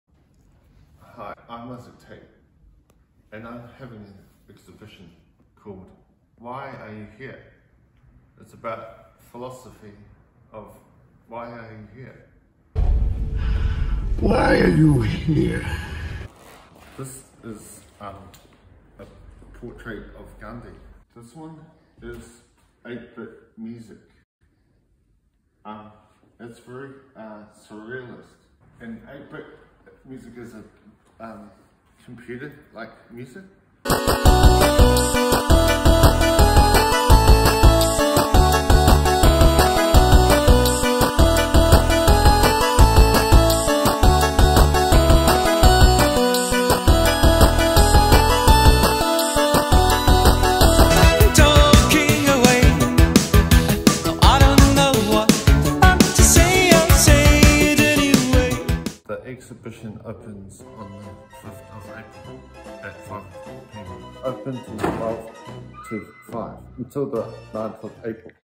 Isaac Tait - Why Are You Here? - Exhibition Trailer
Isaac Tait's exhibition WHY ARE YOU HERE? opens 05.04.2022 at Art Hole, 336 St. Asaph Street.